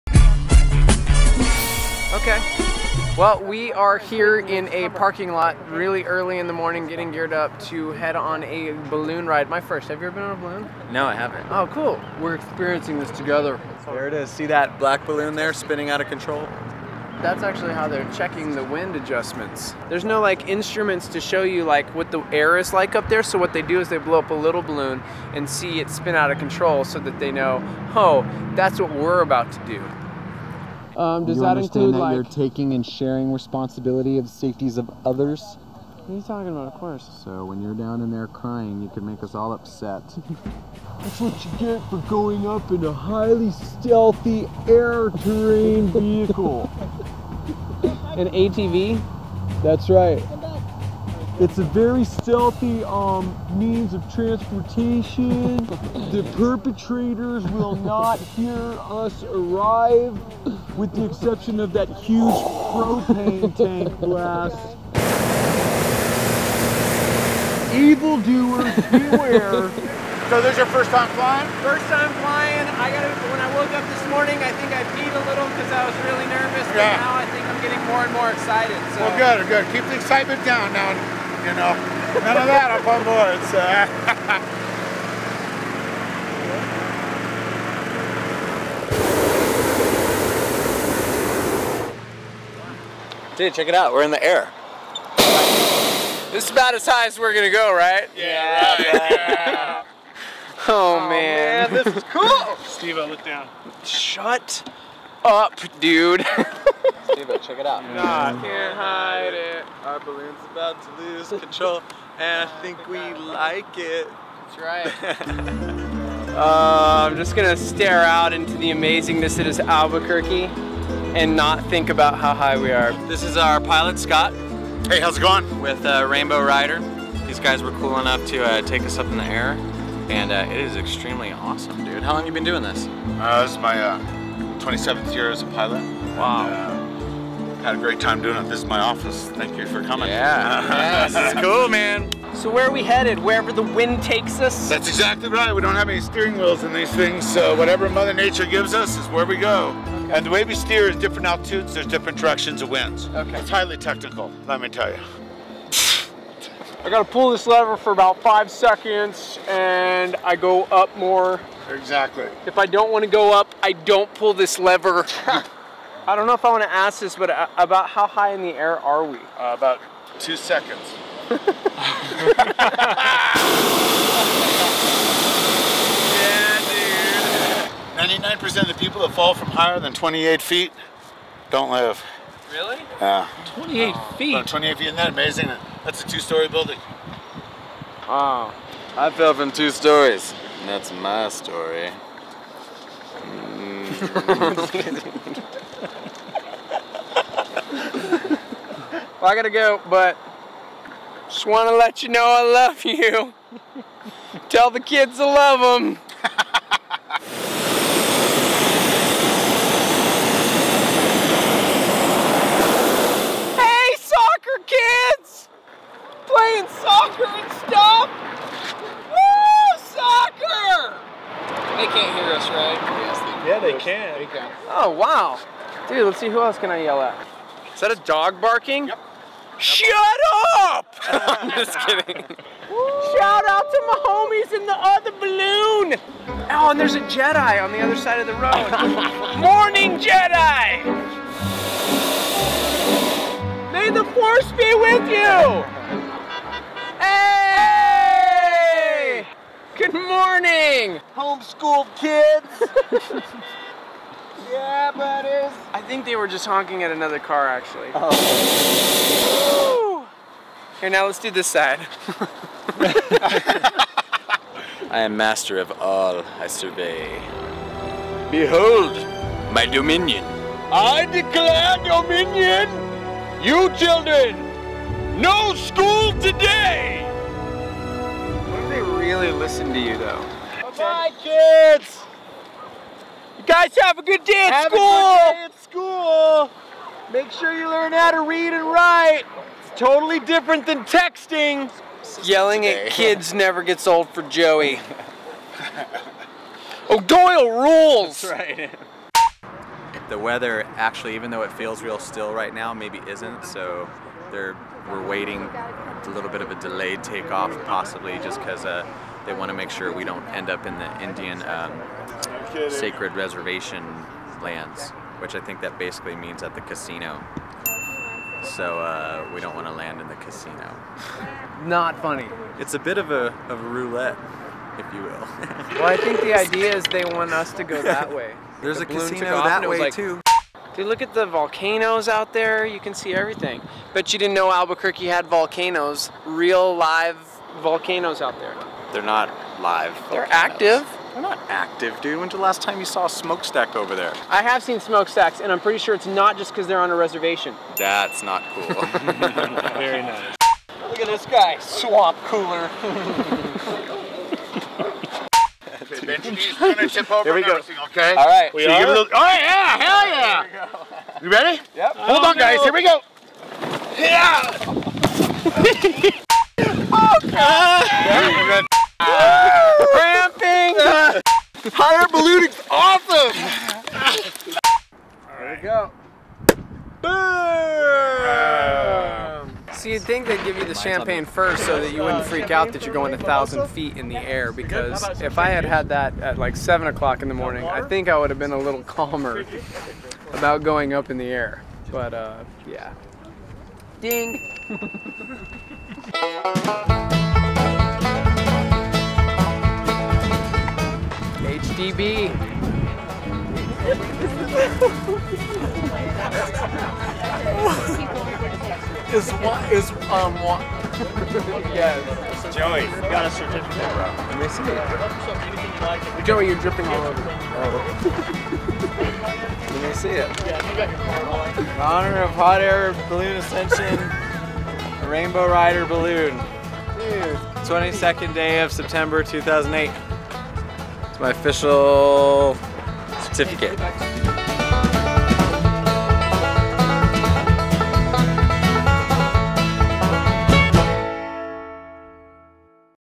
Who wants a moustache balloon ride?
uRock takes to the air once again in a hot air balloon with Scott Appelman from Rainbow Ryders. They pass the time a few hundred feet up by yelling at people on the ground. Also, see what happens when you mix alcohol with stomach bypass surgery.